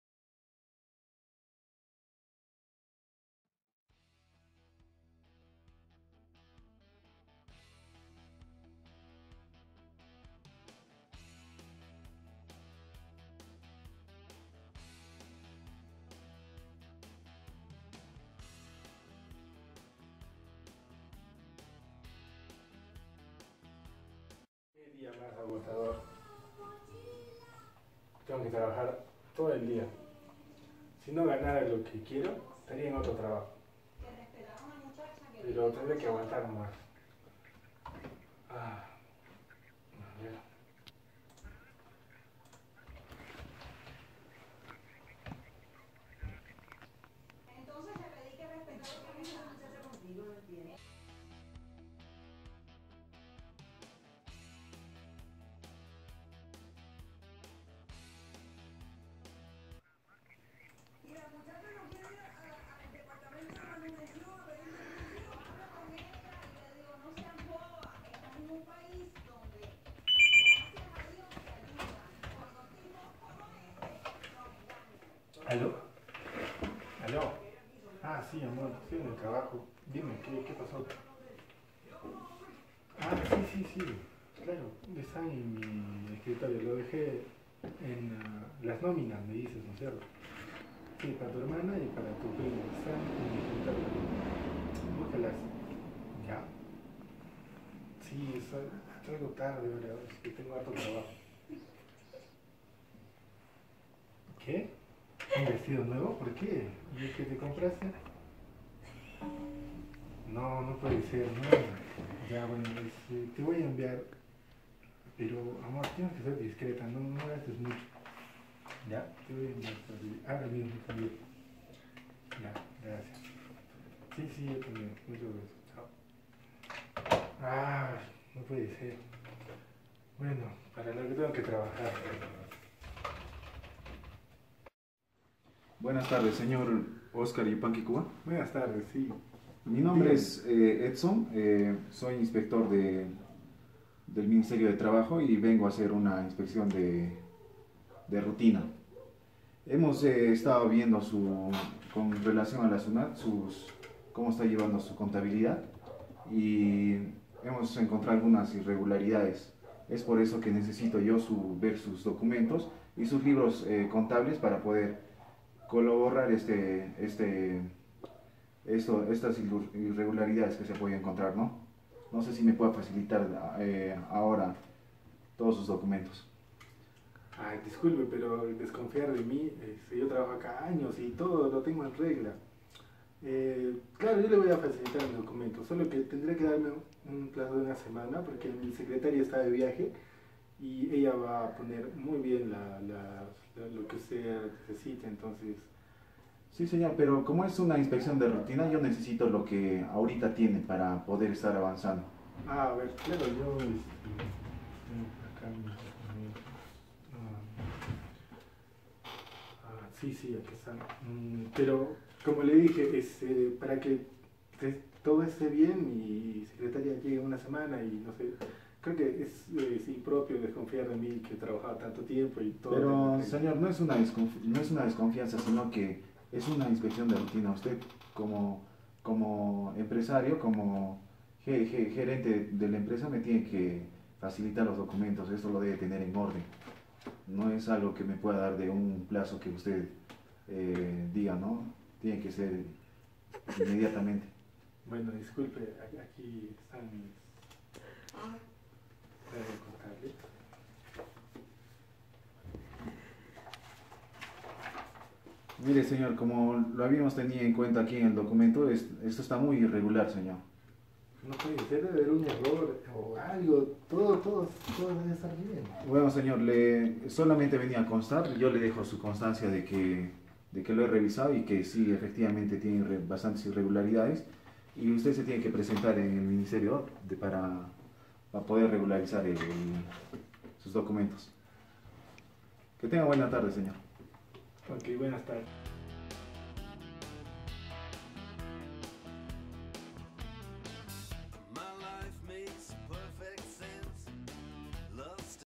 sketch de êtica UAC
sketch por Oscar Yupanqui Cuba

descripción
Ética profesional en la ingeniería de sistemas
Como buen profesional un ingeniero de sistemas debe anteponer su formación y valores morales a sus acciones en la empresa
En caso del manejo financiero y de recursos el ingeniero tiene tanto moral como legal el no hacer uso indebido de los fondos de la empresa donde labora